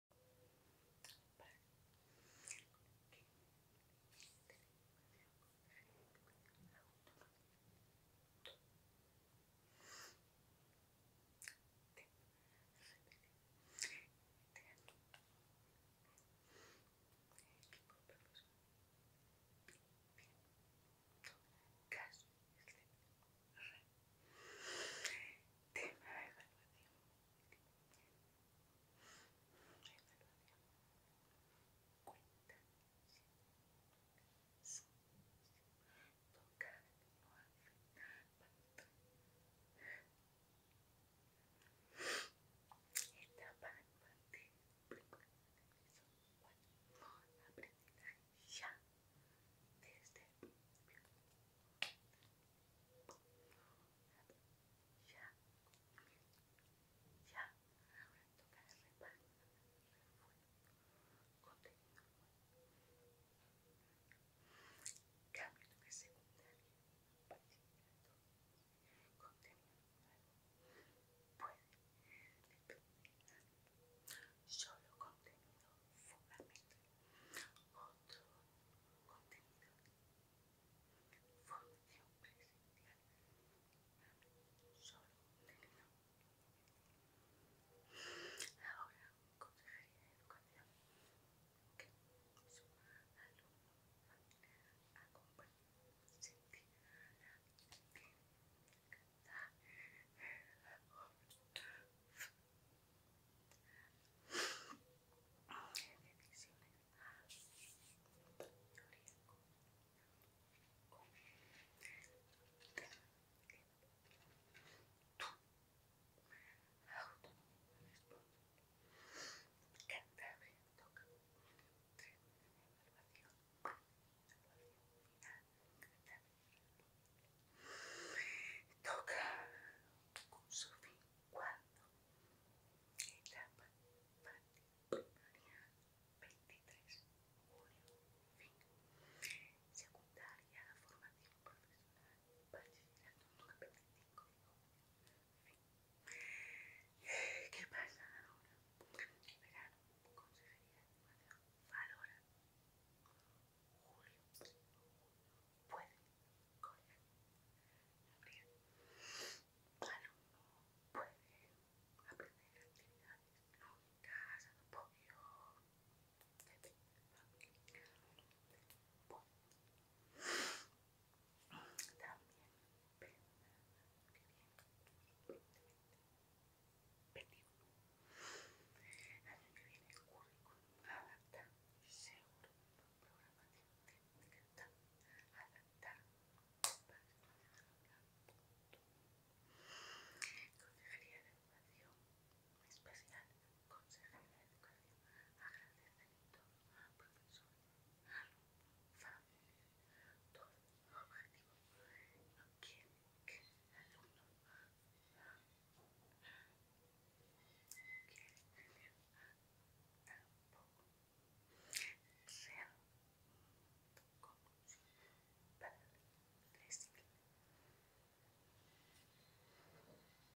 Información sobre la última conferencia sectorial de educación (15 abril 2020)
Cristina Assal, asesora educativa, nos explica en LSE, la situación actual en el ámbito educativo tras la reunión mantenida el pasado 15 de abril.